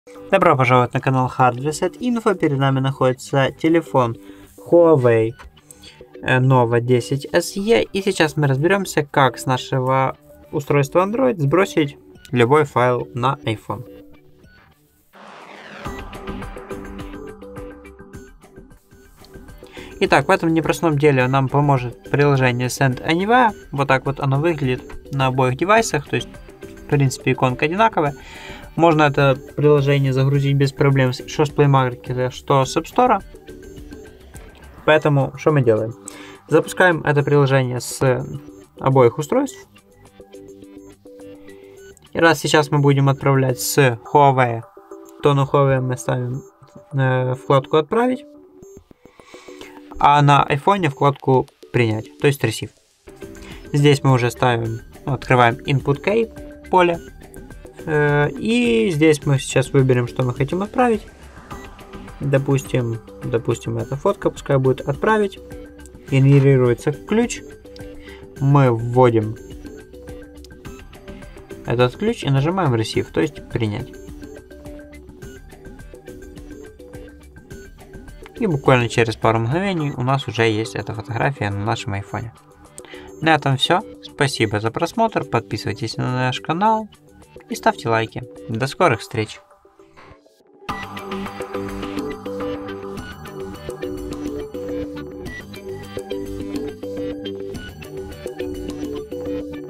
HUAWEI Nova 10SE | Как перенести файл с айфона на устройство HUAWEI Nova 10SE
В этом видео мы покажем, как на устройтсво HUAWEI Nova 10SE можно перенести файл с айфона. Если вы хотите перекинуть файл с айфона на HUAWEI Nova 10SE, но не знаете что для этого нужно, то это видео для вас. Сделайте все так, как показано в этом видео, чтобы перенести файл с айфона на HUAWEI Nova 10SE.

Как перенести файл с айфона на HUAWEI Nova 10SE? Можно ли перенести файл с айфона на HUAWEI Nova 10SE?